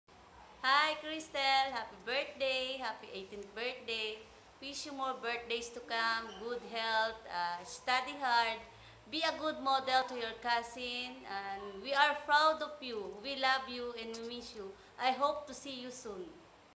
Tita Claire's Birthday Message to Khrystle
This is Tita Claire's Birthday Message for Khrystle's Debutante Ball.